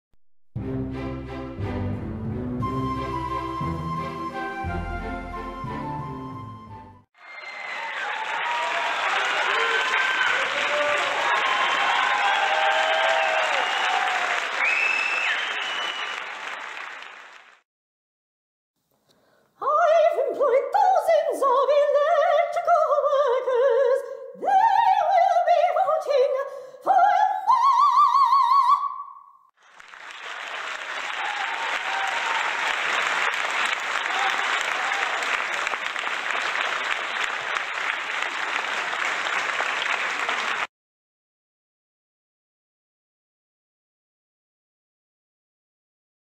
April 5, 2019 (Electrical Workers): Donald Trump Tweet Song Opera Sock Puppet
Original Tweet: I’ve employed thousands of Electrical Workers. They will be voting for me!